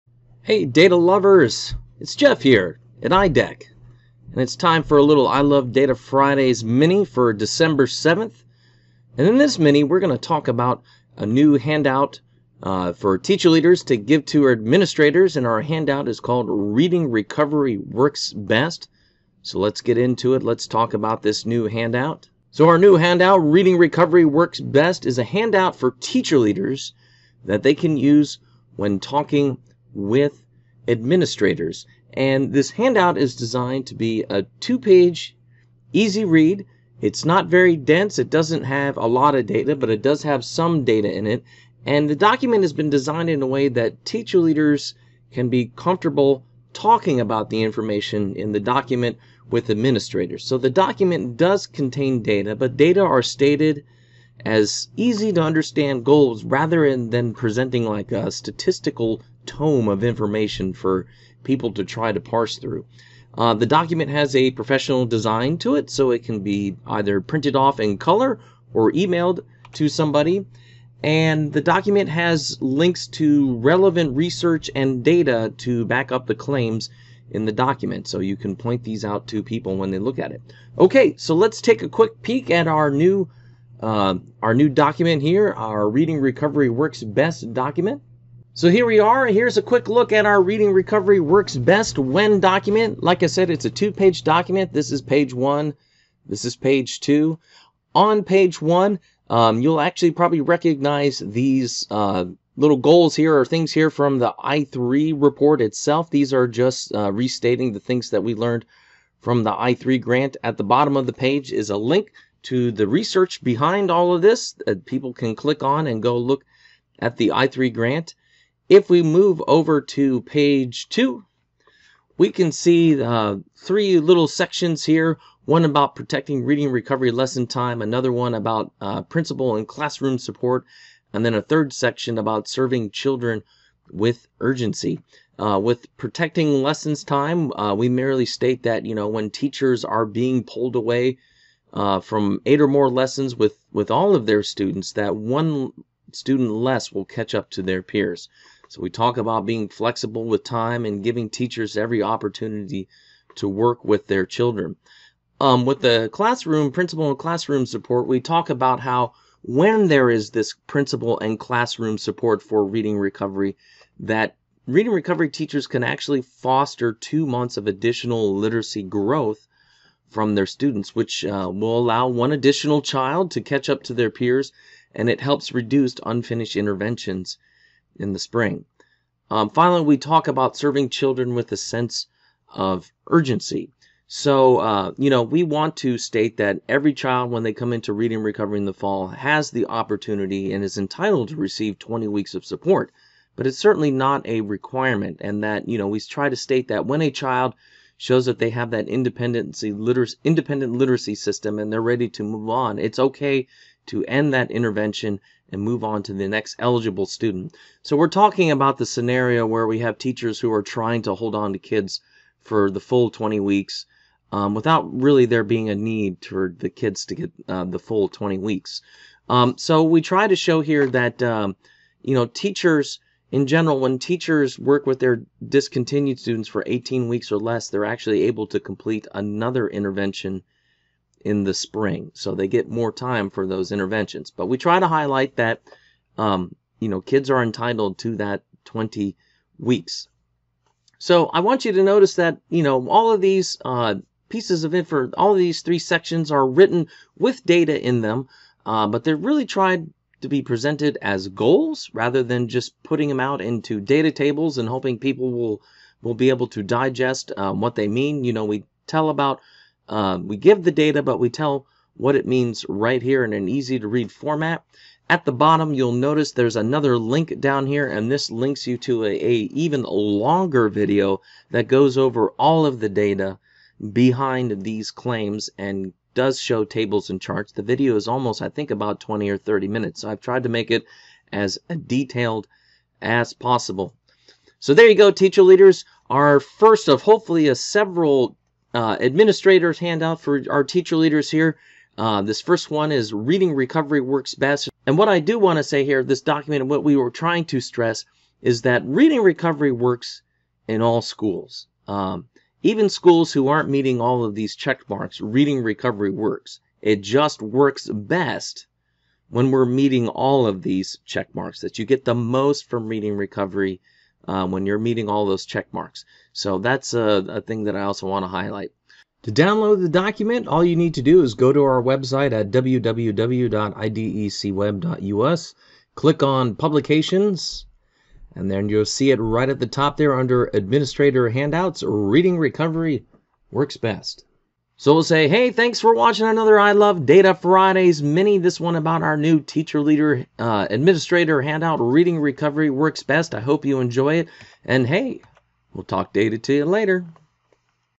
ILDF - Mini - Administrator Handout - Reading Recovery Works Best
In this mini we talk about the a new handout for Teacher Leaders that can used when talking to administrators.